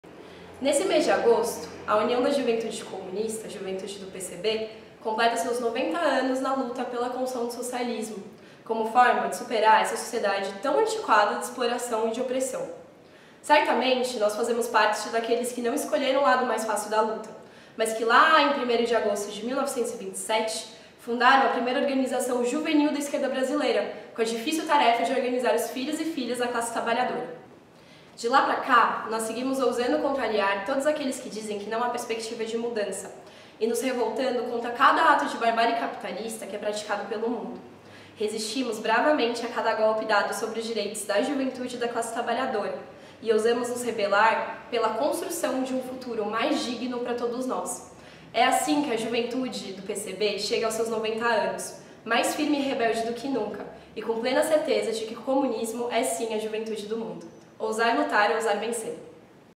90 anos de UJC Brasil: Lígia Fernandes - Coordenação Nacional da UJC Brasil
Nesse mês de agosto, a União da Juventude Comunista (UJC-Brasil) juventude do PCB, completa os seus 90 anos na luta pela construção do socialismo como forma de superar essa sociedade tão antiquada de exploração e opressão.

Certamente nós fazemos parte daqueles que não escolheram o lado mais fácil da luta, mas lá em 1 em 1º de agosto de 1927 fundaram a primeira organização juvenil da esquerda brasileira com a difícil tarefa de organizar os filhos e filhas da classe trabalhadora.

De lá para cá, nós seguimos ousando contrariar todos aqueles que dizem que não há perspectiva de mudança. E nos revoltamos contra cada ato de barbárie capitalista que é praticada pelo mundo.

Resistimos bravamente a cada golpe dado sobre os direitos da juventude da classe trabalhadora e ousarmos nos rebelar por um futuro mais digno para todos nós.

É assim que a juventude do PCB chega aos seus 90 anos. Mais firme e rebelde do que nunca e com plena certeza que o Comunismo é sim a juventude do mundo.

Ousar Lutar, ousar Vencer!